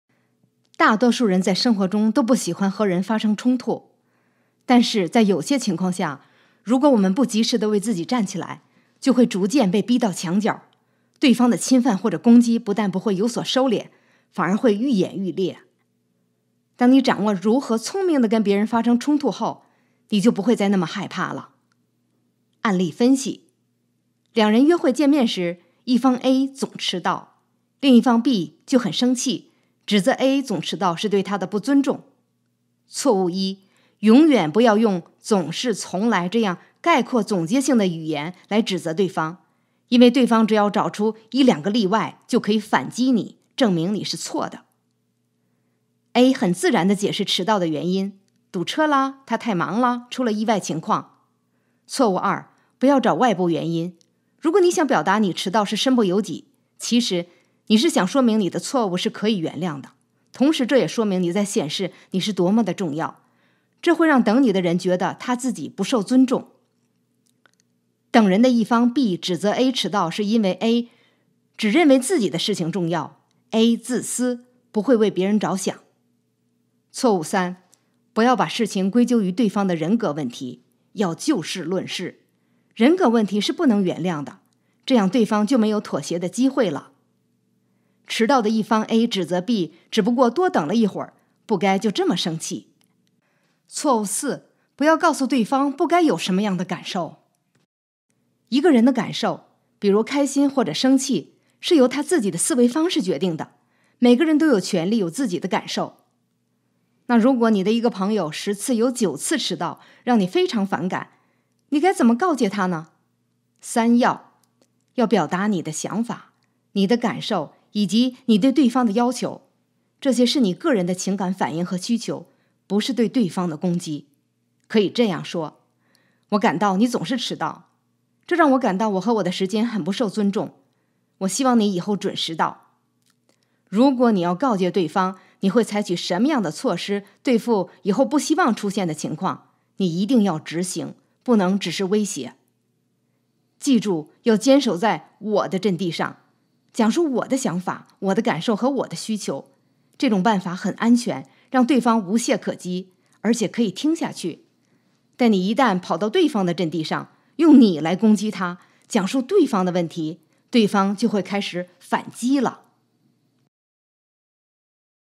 怎样聪明地和人发生冲突 不要等被逼到墙角 反击 心理学
运用3个要，4个不要，对方就不会有反击的机会，而且能够听下去你的论点。一般人最犯的错误：指责对方总这样或那样，对方只要拿出一个反例，你的观点就会被无效否定了。如果你要为自己辩解，错误的方式会让人觉得你认为你的做法是可以原谅的，你非常以自我为中心，如何用正确的方式解释，让对方能够倾听是有技巧可循的。 实用